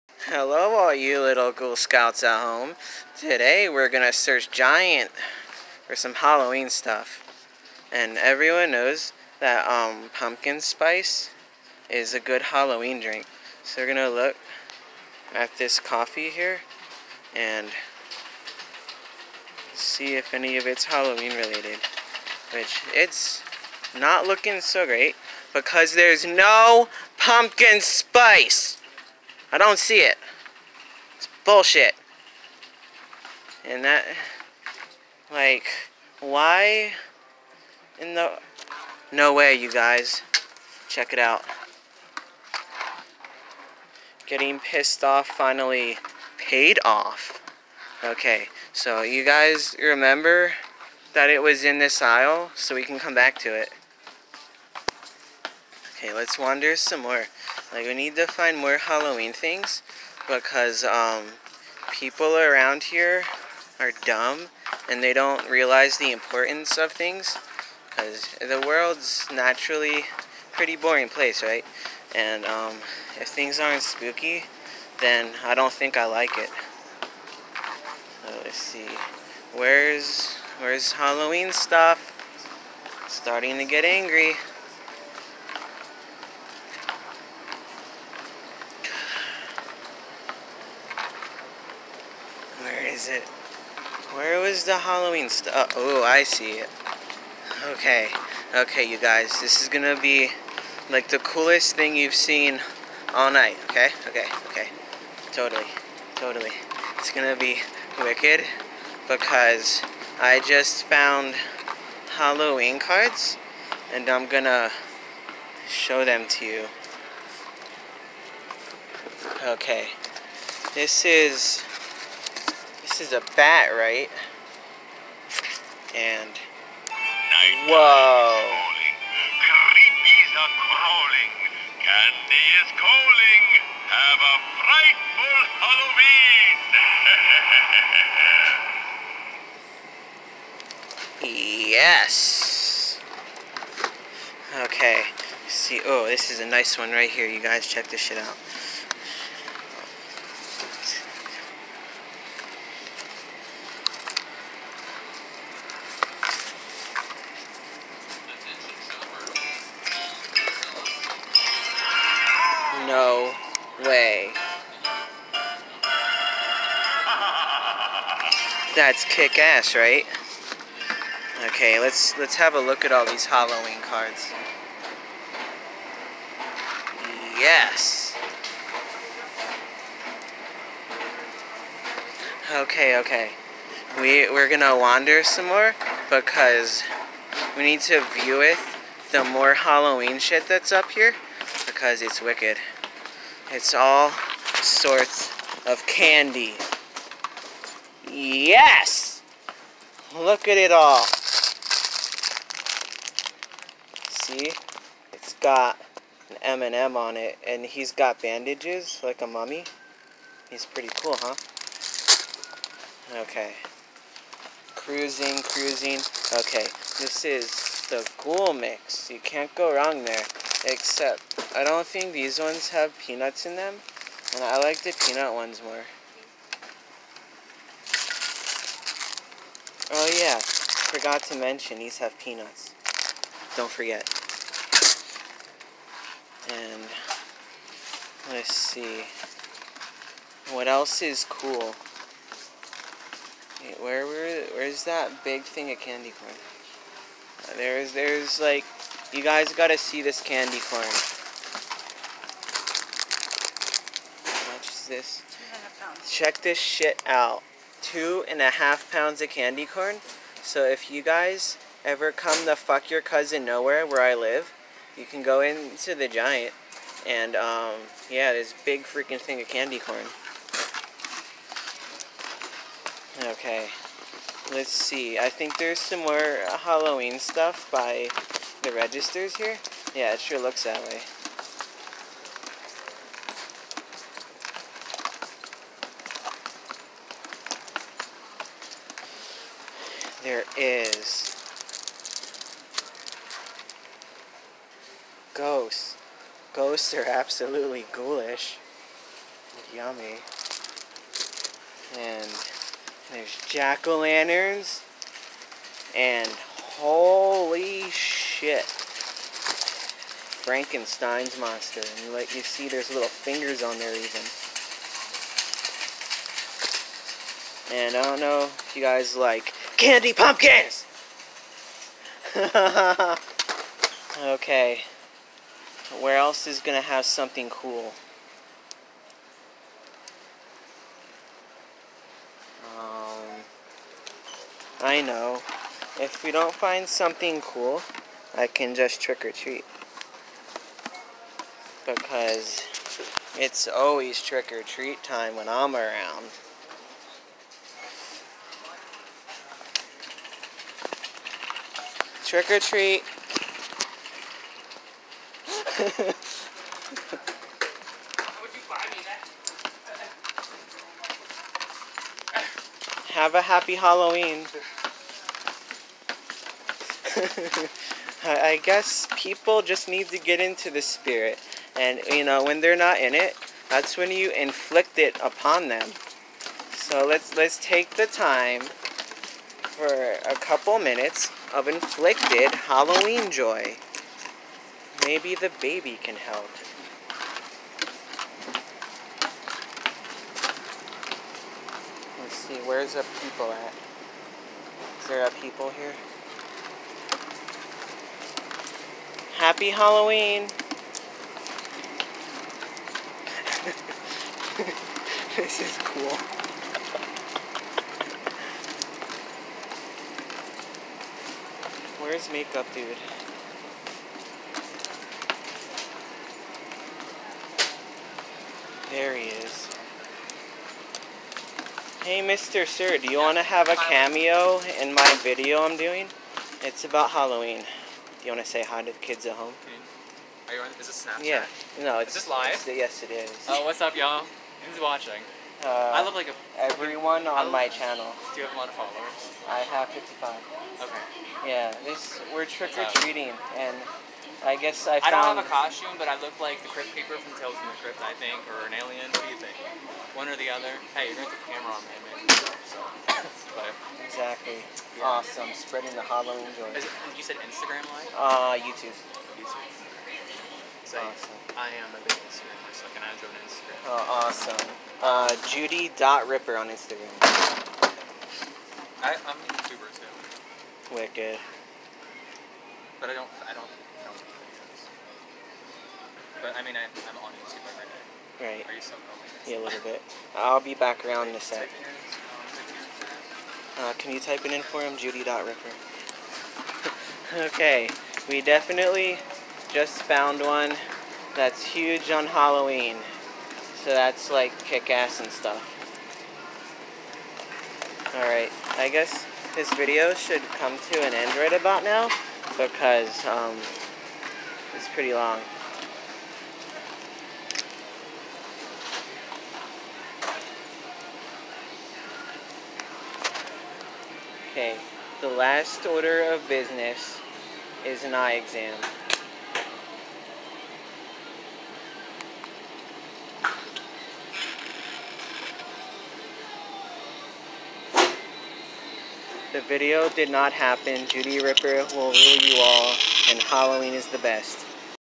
B-Movie in Giant
Wandering around Giant and angering normal people is cool. Plus, I'm a freak and that's what we do. Be ourselves and anger normal people!